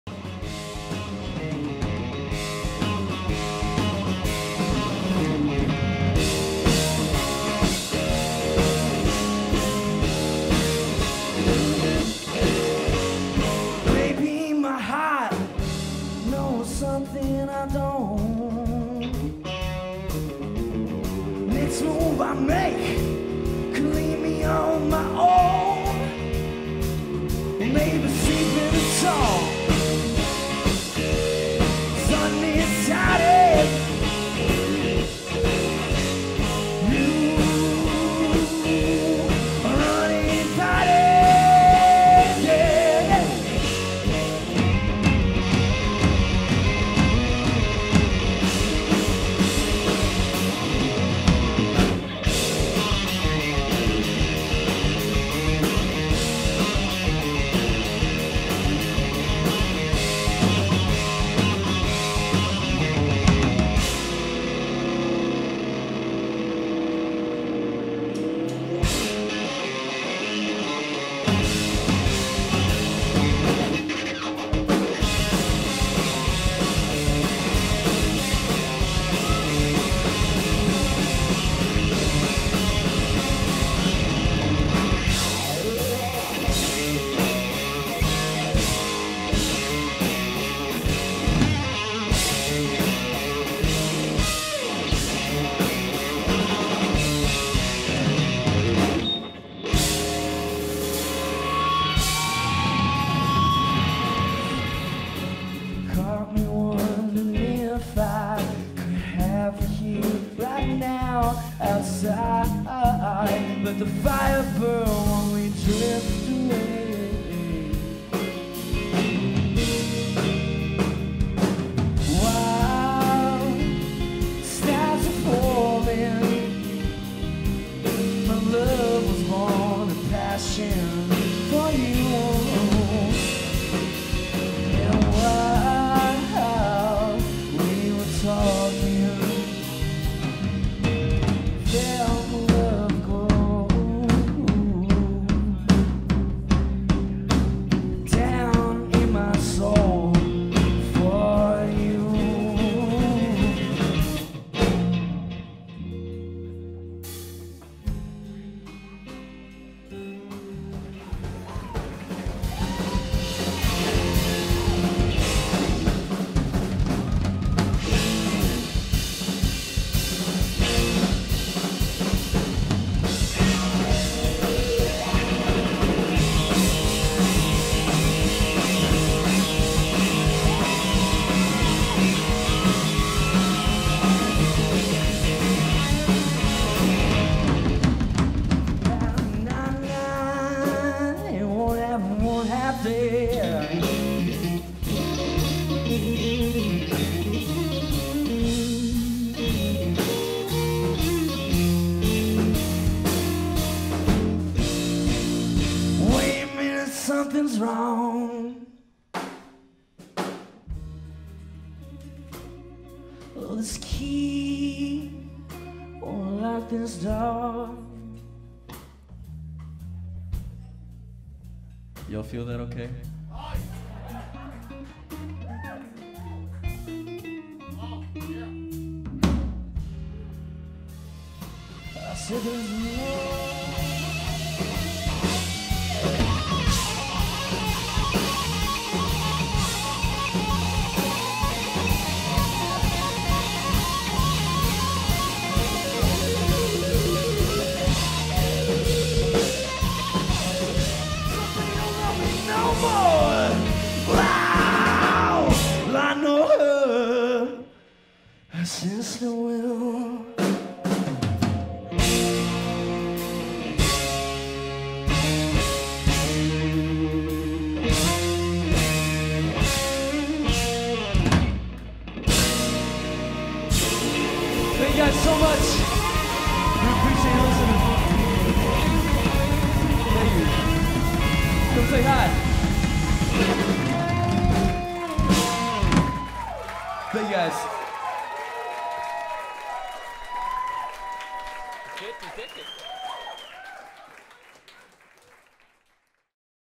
Clay Melton Live @ Dosey Doe March 2018
You ever experience the energy and excitement of a Clay Melton Music concert in person? Here’s what you’re missing. Check out live footage from the March 2018 show at Dosey Doe in The Woodlands, TX
Video by: Brandon Bond